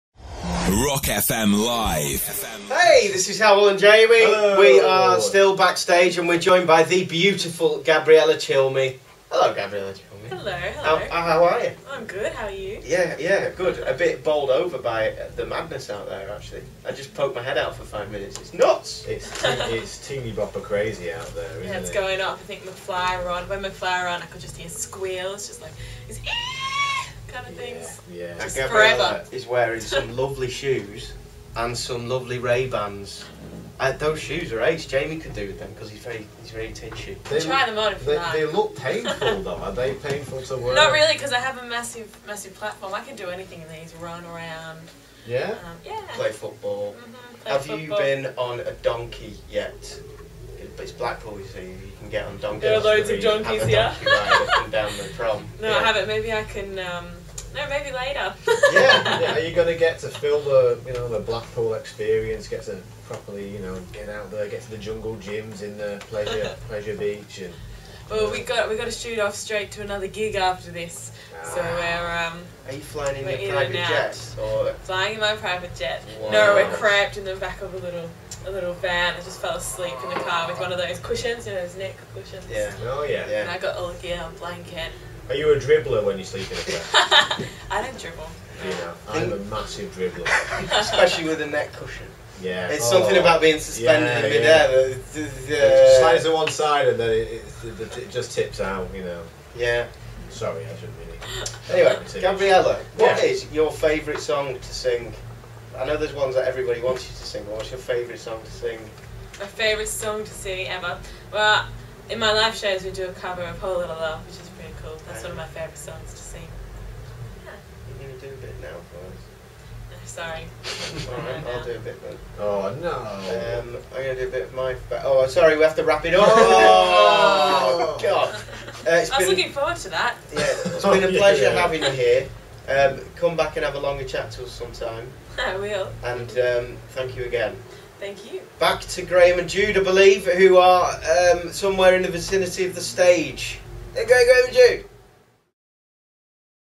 Rock FM LIVE!: Gabriella Cilmi
Gabriella Cilmi interviewed backstage, by Hywel and Jamie, at Rock FM LIVE!